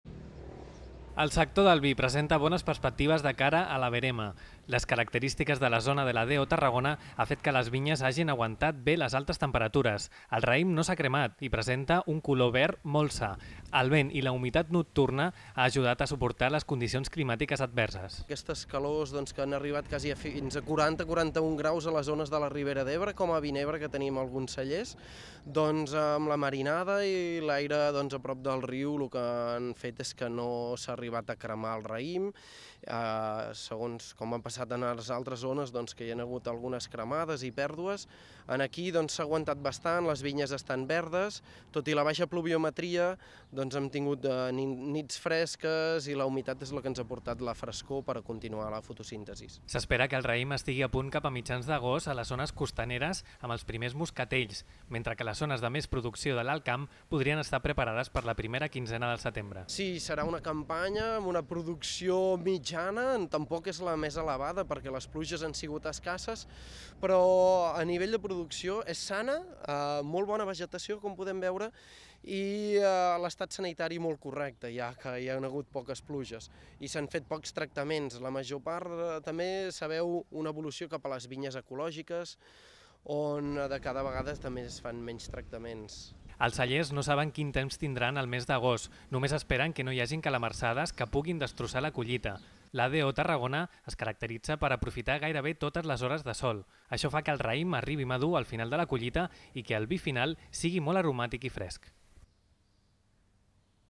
Les vinyes de la DO Tarragona encaren la verema amb bones expectatives
S'espera que la collita sigui bona tot i les condicions de calor extrema viscudes en les darreres setmanes.